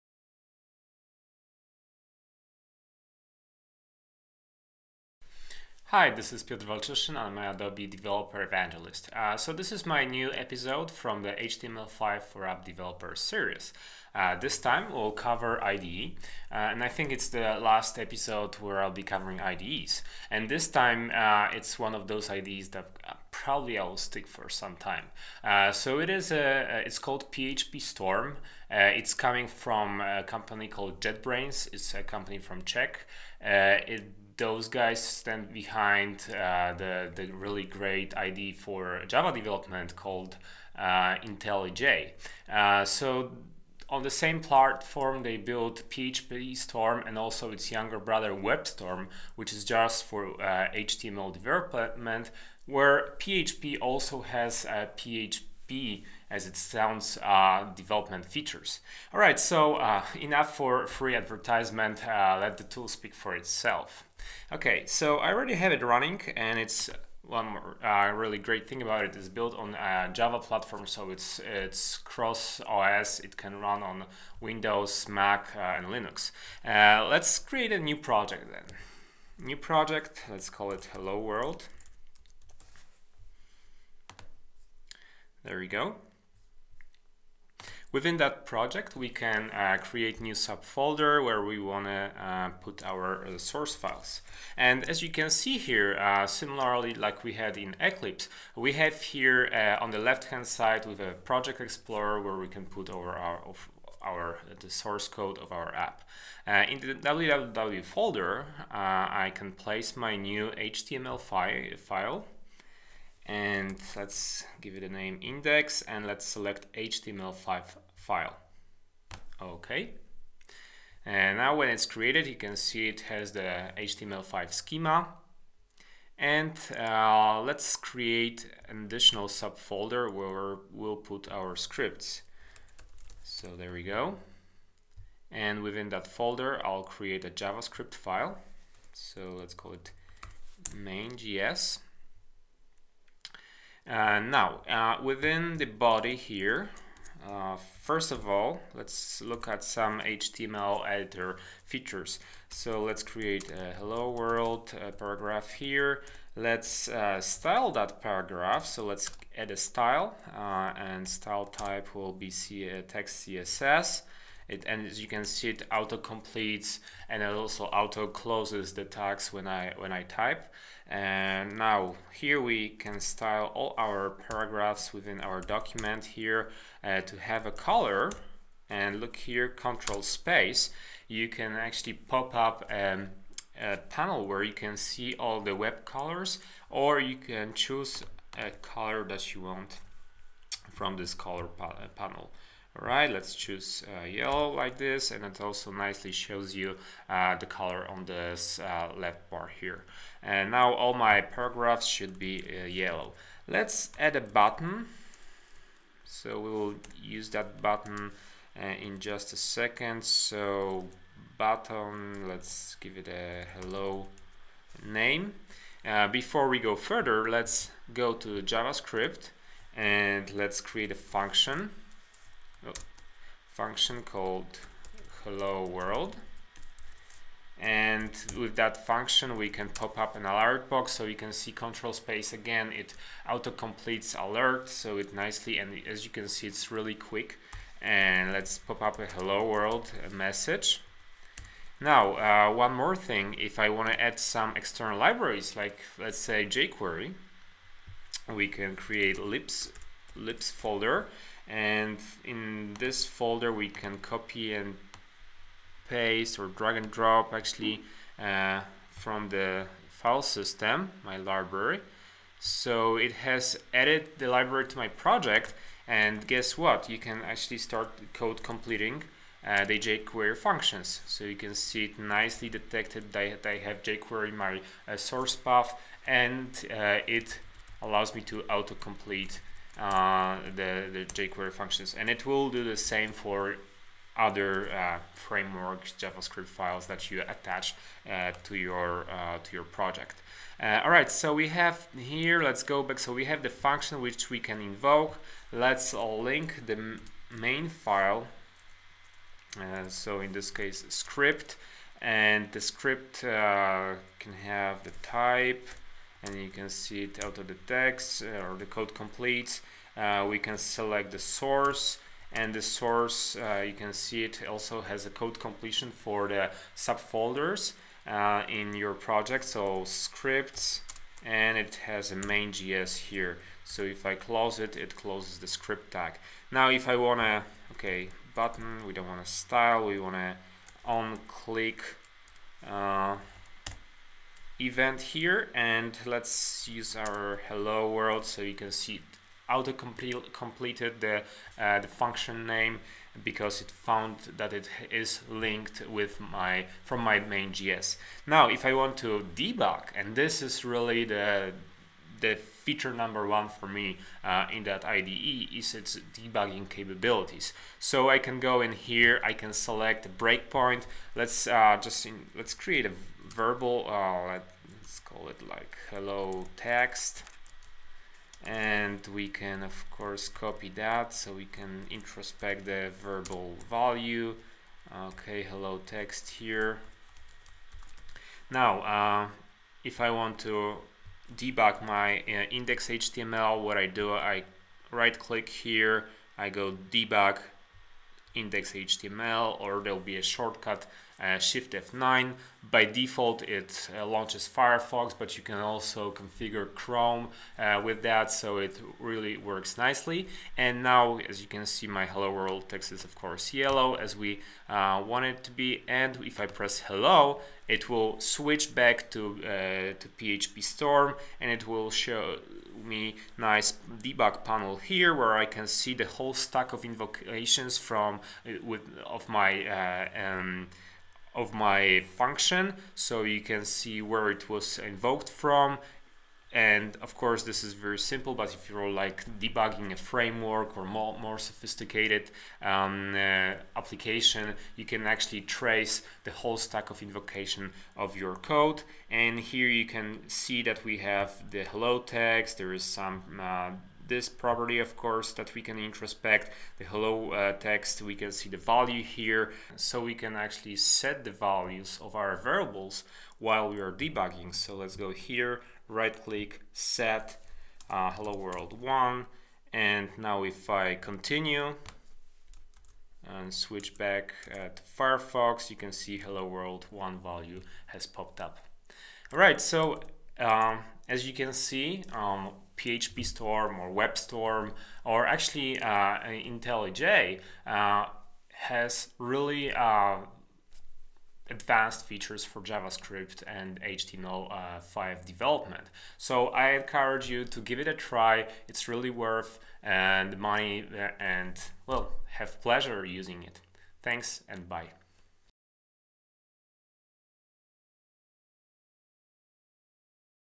HTML5 for App Developers: PhpStorm/WebStorm
This is my fourth video tutorial in the HTML5 for App Developers series. This time I'm reviewing PhpStorm/WebStorm IDE that really eases JavaScript/HTML5/CSS3 development and makes it really fun!